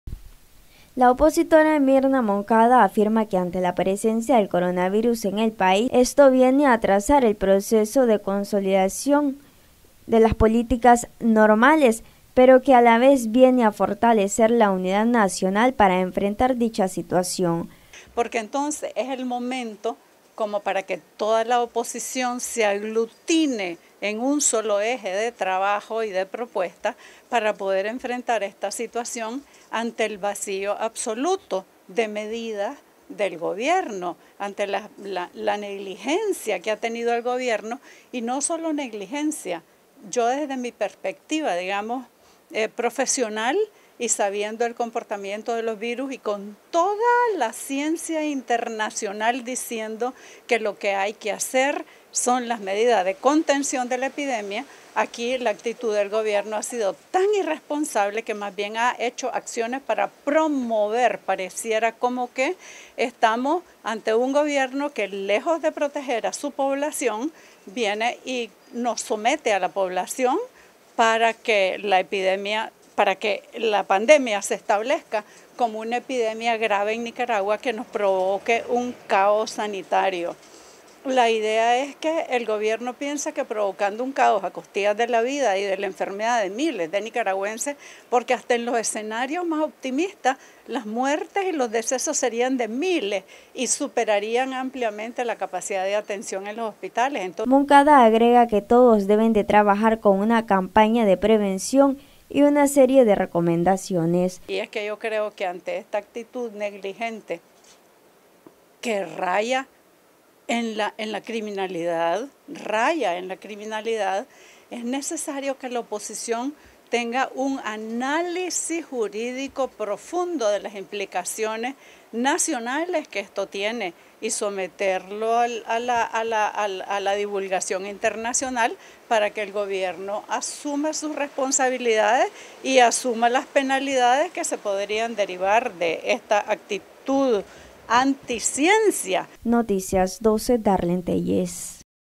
Coronavirus mostrará la unidad de los nicaragüenses para enfrentar la enfermeda
La opositora Mirna Moncada dio a conocer que presencia del Coronavirus viene a fortalecer   a un más la unidad de los nicaragüenses para enfrentar emergencia.